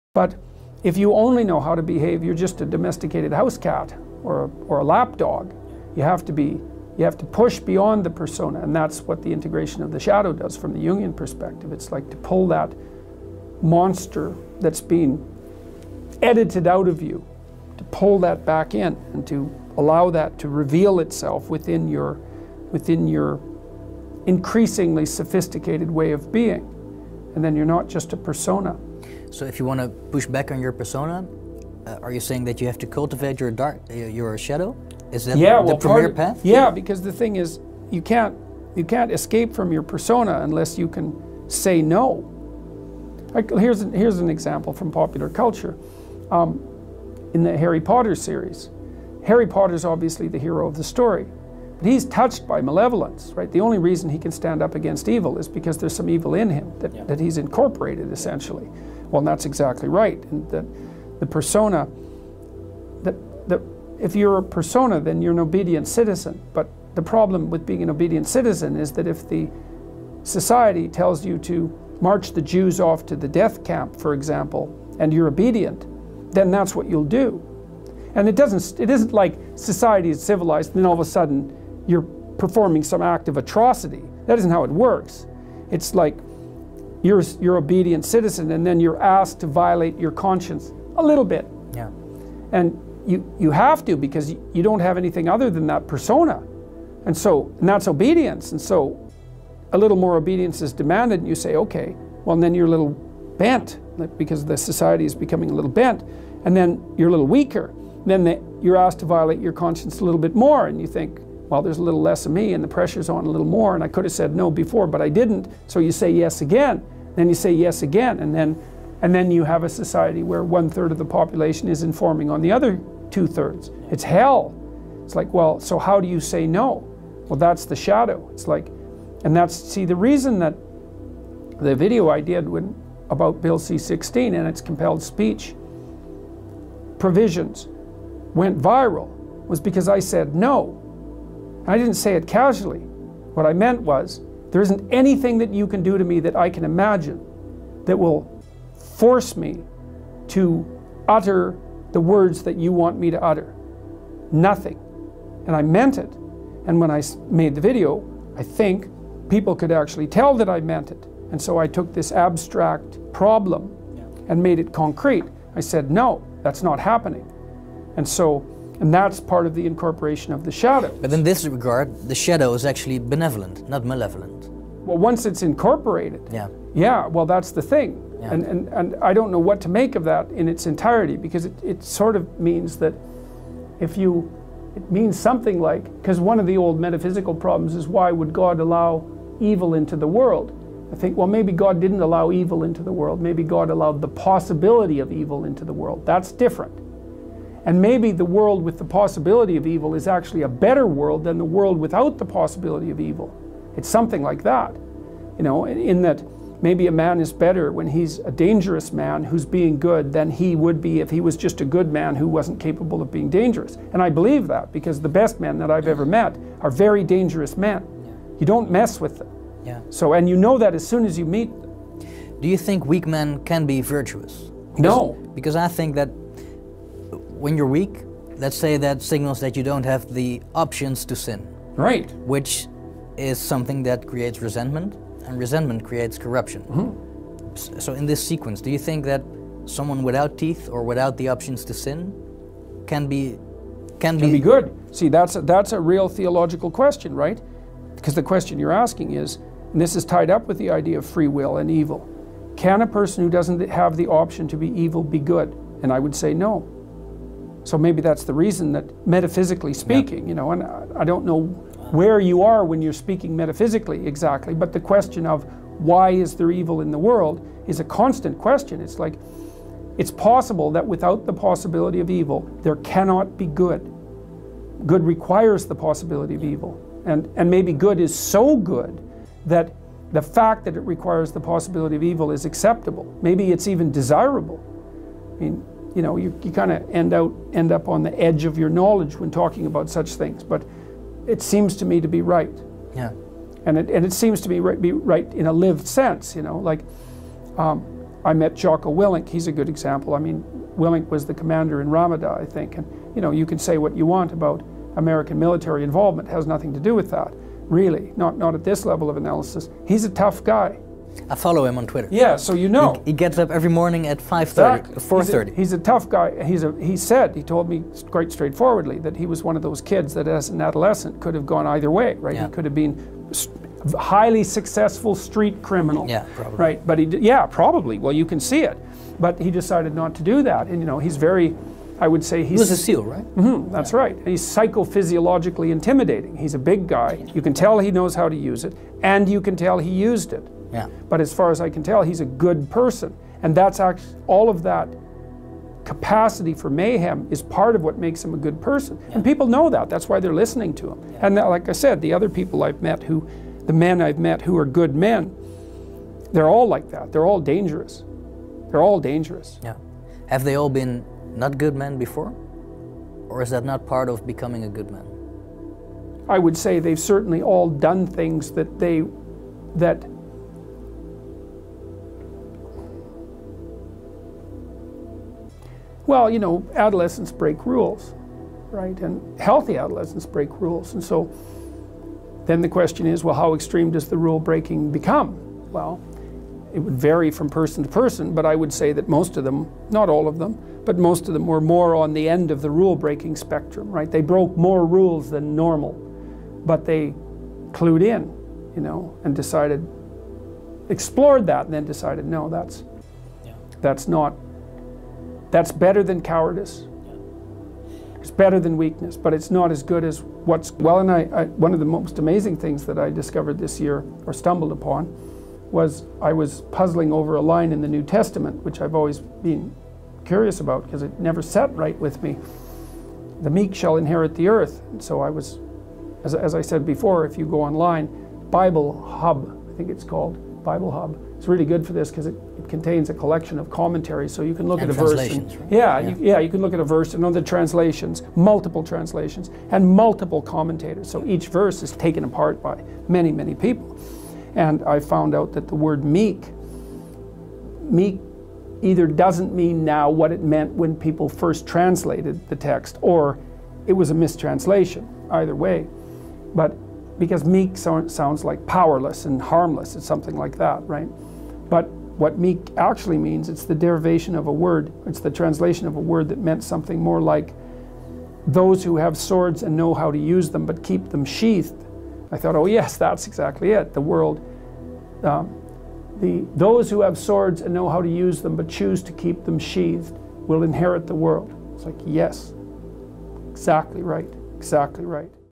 Jordan Peterson - BE DANGEROUS [BUT DISCIPLINE ]
Be dangerous, but disciplined, that is the way to move forward. It's better to be dangerous, yet disciplined than to be harmless, because once you are disciplined, then you respect yourself that you can do a lot of things, but you choose to do good!

If you only know how to behave, you're just a domesticated house cat or a lap dog, you have to be, you have to push beyond the persona. And that's what the integration of the shadow does from the union perspective, it's like to pull that monster that's been edited out of you to pull that back in, and to allow that to reveal itself within your increasingly sophisticated way of being. And then you're not just a persona.

You can't escape from your persona. Unless you can say no. here's, here's an example from popular culture. In the Harry Potter series, Harry Potter is obviously the hero of the story. He's touched by malevolence, right? The only reason he can stand up against evil is that there's some evil in him, that he's Incorporated, essentially. Well, that's exactly right, that the persona, that if you're a persona, then you're an obedient citizen. But the problem with being an obedient citizen is that if society tells you to March the Jews off to the death camp, for example, and you're obedient, then that's what you'll do. And it doesn't, it isn't like society is civilized, then all of a sudden, you're performing some act of atrocity. That isn't how it works. It's like, you're an obedient citizen, and then you're asked to violate your conscience a little bit. Yeah. And you have to because you don't have anything other than that persona. And so that's obedience.

All the videos uploaded on this channel are just to motivate you so you don't give up and become successful in life.